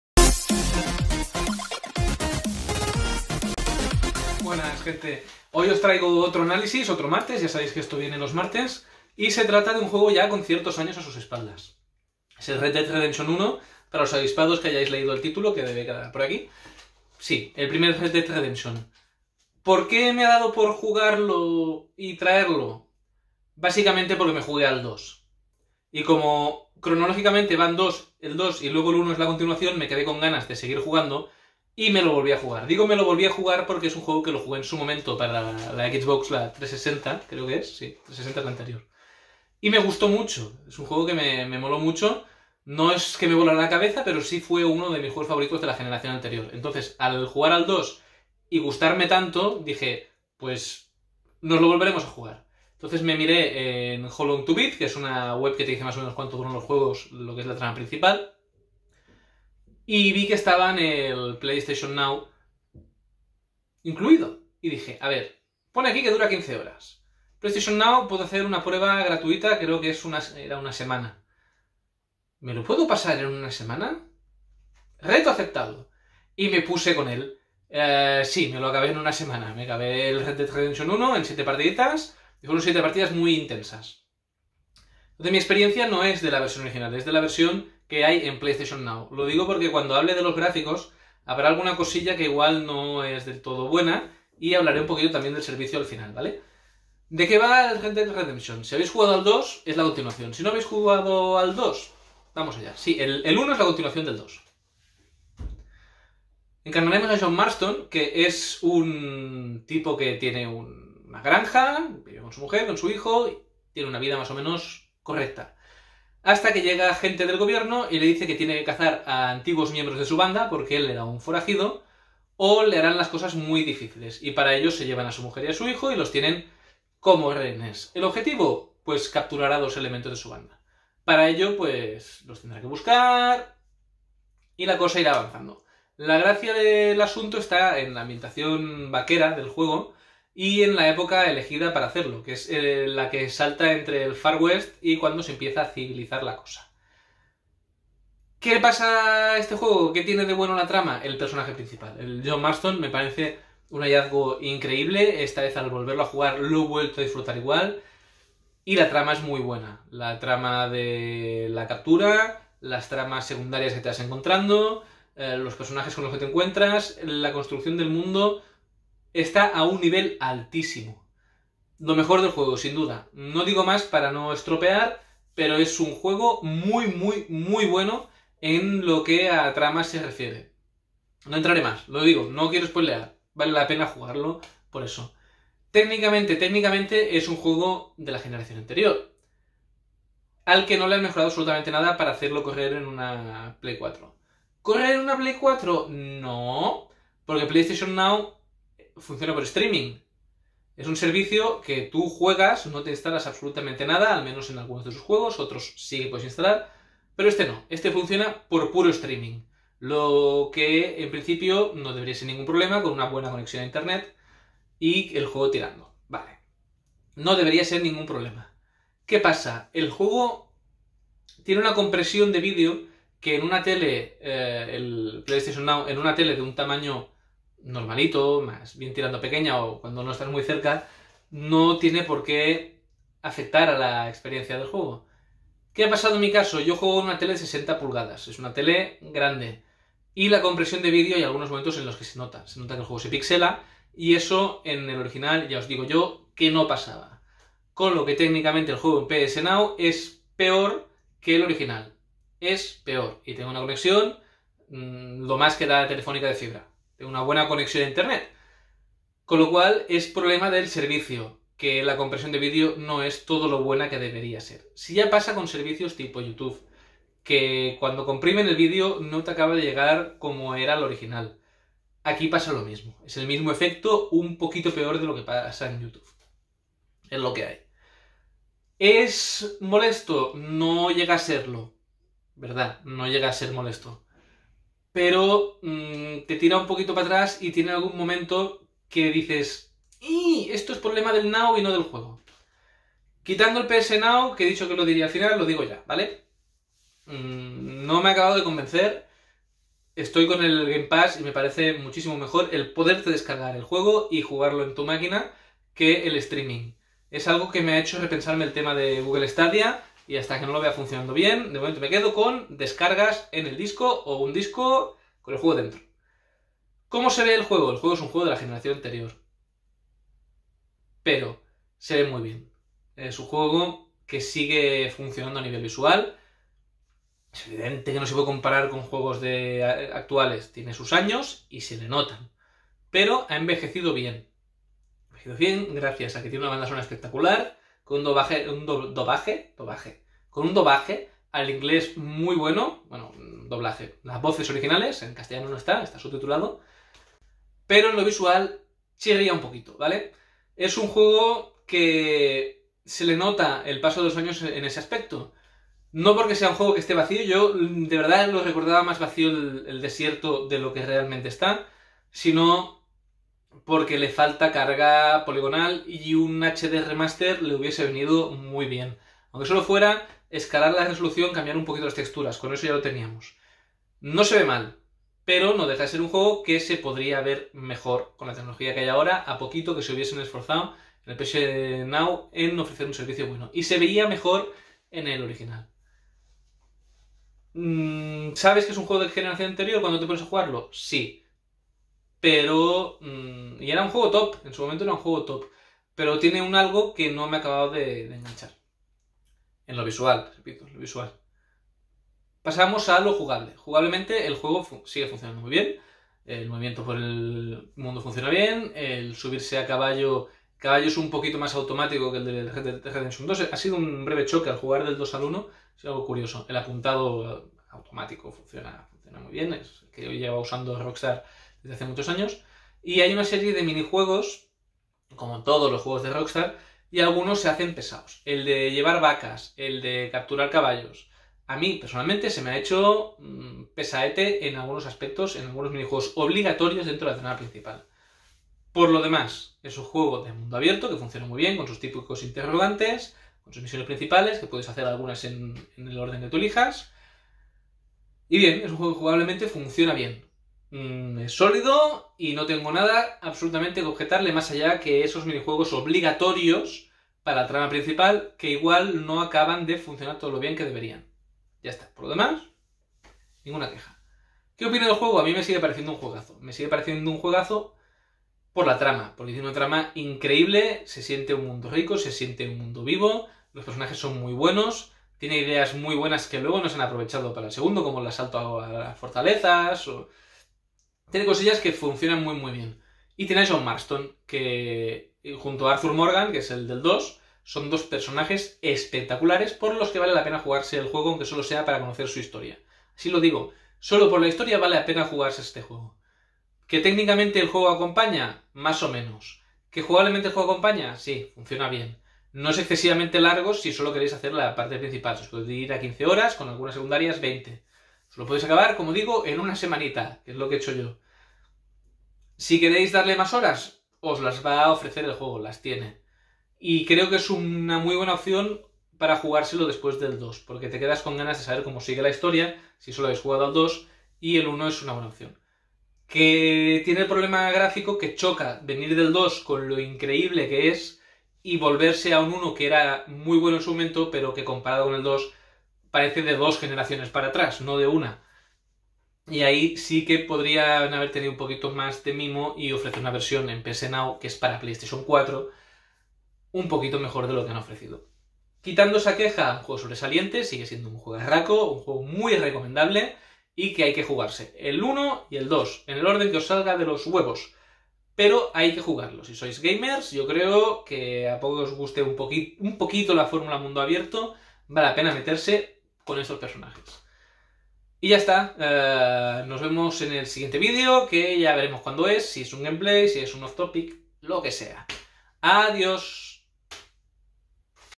Red Dead Redemption (2010) | Análisis
Después de jugarme RDR2 no he podido aguantarme y me he puesto con la primera. Aquí mis conclusiones.